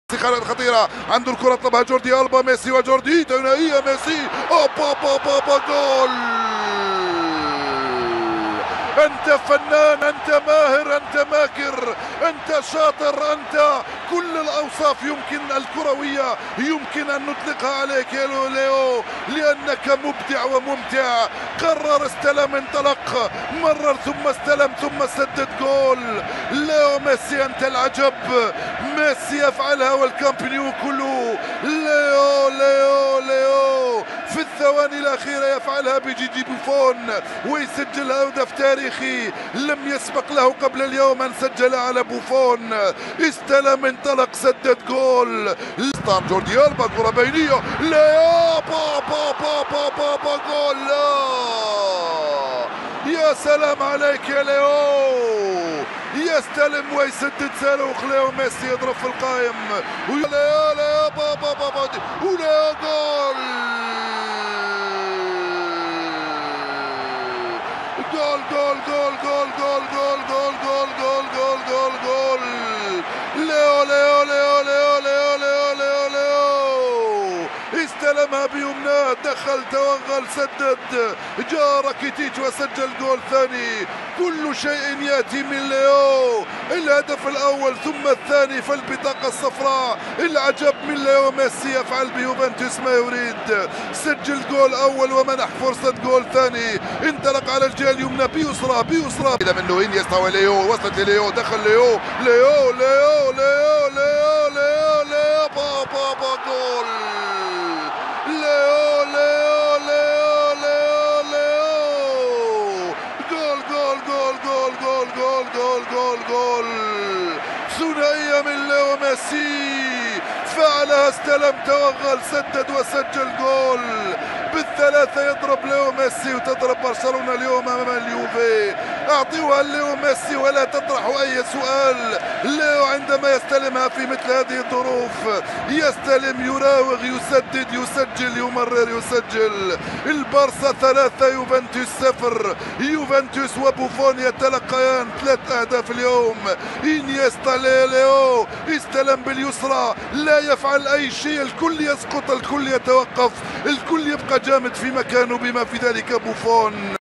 اهداف برشلونة و جوفنتيس 3-0 //هدفين لميسي وهدف راكيتيتش// دورى ابطال اوروبا 12/09/2017
اهداف  برشلونة و جوفنتيس 3-0  دورى ابطال اوروبا 12/09/2017